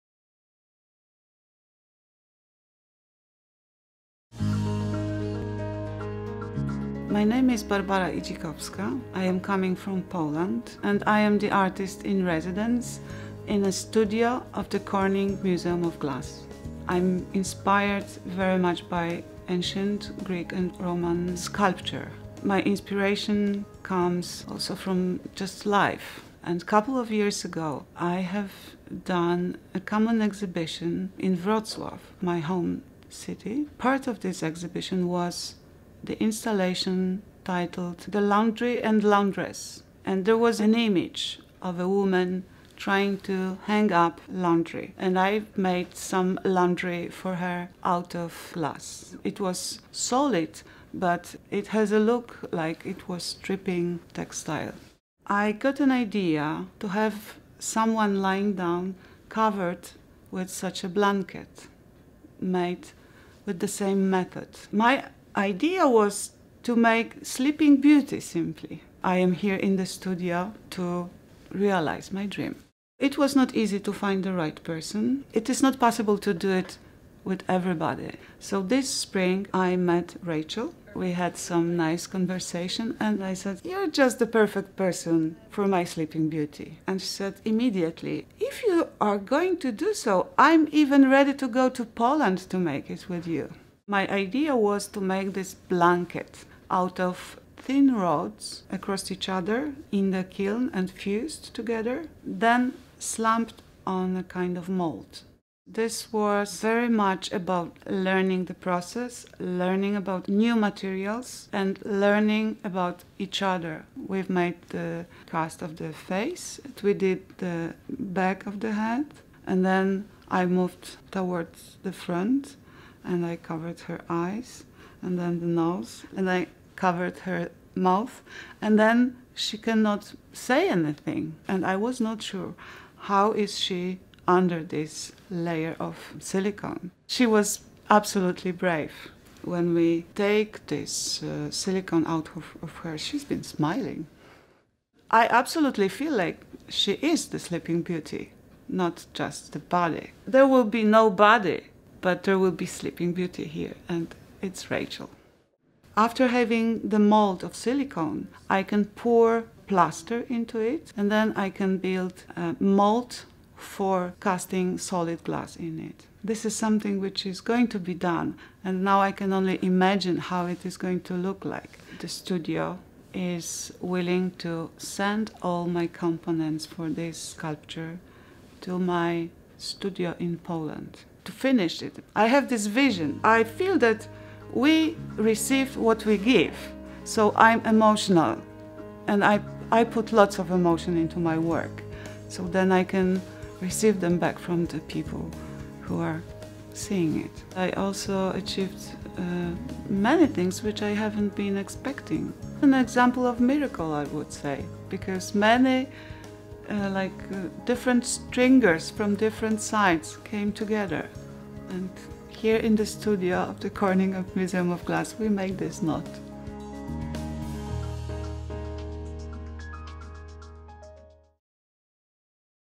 Artist-in-Residence: Barbara Idzikowska
Barbara Idzikowska has been fascinated by glass for over twenty years. Living and working in Wrocław, Poland, she seeks to give ideas material form through her work in glass. In 2004, she established Atelier Si, Pb..., a studio of drawing on glass, with the graphic designer Eugeniusz Get Stankiewicz. Idzikowska has presented solo exhibitions since 1991, including the most recent Laundry and Laundress at the Muzeum Karkonoskie in Jelenia Góra, Poland, in 2012. Her work has been included in New Glass Review 19, 18, and 15.

At Atelier Si, Pb..., she sought to find new approaches for glass art in architecture. Projects include the installation for the Town Hall in Wrocław and The Dürers of the Lubomirski Princes, a monumental drawing in glass installed as a window at the Ossoliński National Institution in Wrocław.

Idzikowska came to The Studio in October 2013 as part of the joint Artists-in-Residence partnership with the John Michael Kohler Arts Center in Sheboygan, WI. During her residency, she continued exploring the "symbolic, formal, and technological relationships between drawing and glass, space, architecture." "I have experimented with various techniques and made my own technological discoveries,"says Idzikowska. "Clarity and simplicity have been my guiding concerns."